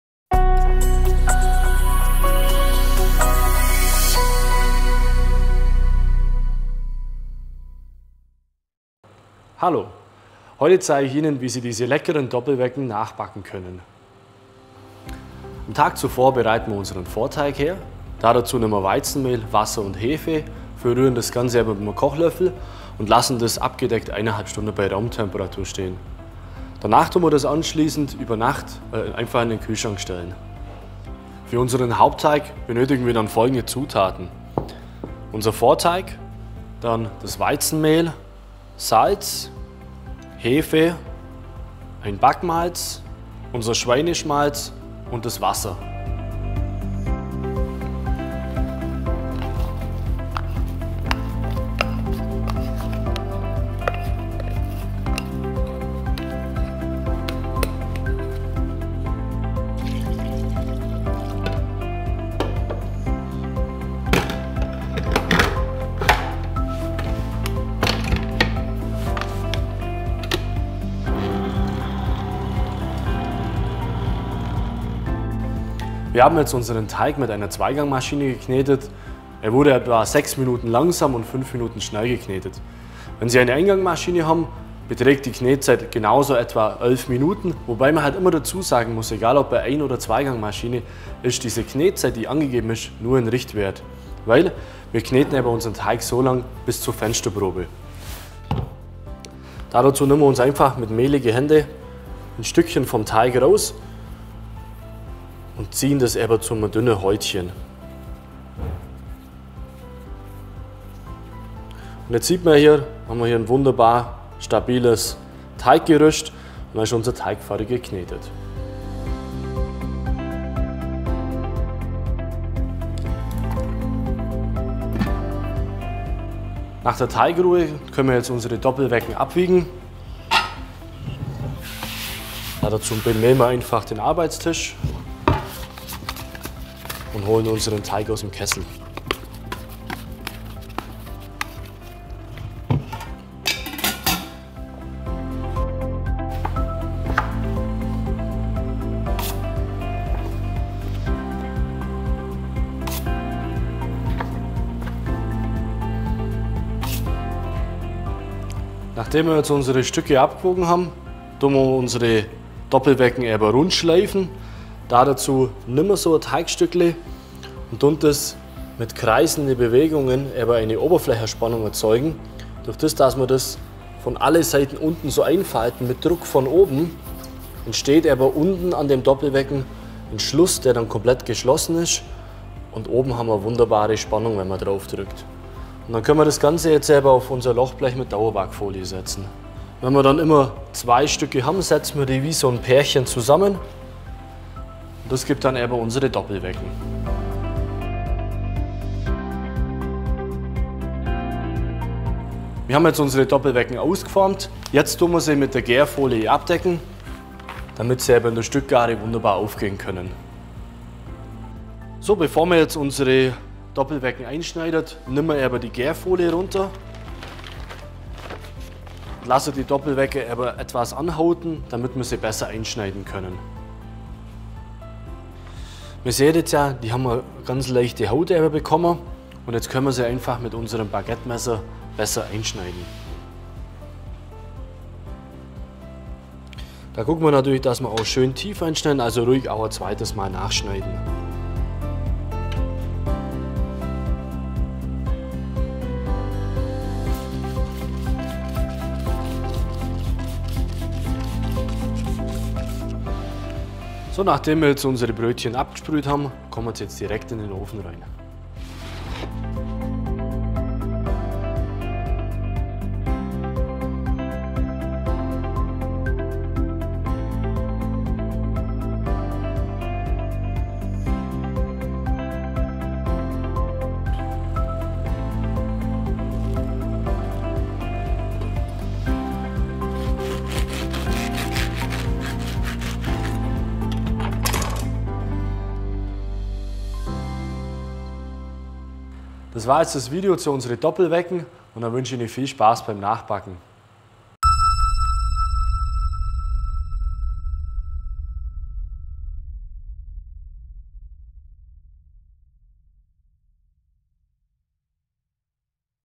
Schwäbische Doppelwecken Selbermachen – Häussler Backdorf
Bäckermeister Stefan Leichtle zeigt Ihnen Schritt für Schritt wie Ihnen leckere Doppelwecken zum Frühstück gelingen. Viel Spaß beim Nachbacken!